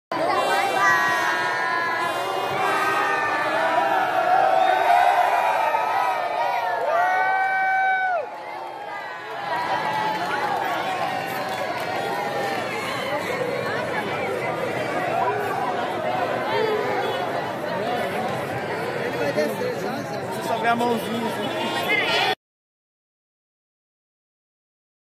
Lula e Janja acenam para apoiadores da varanda de hotel
Quase 200 eleitores do futuro presidente, que tomará posse neste domingo (1º), se aglomeram em frente ao edifício, em Brasília.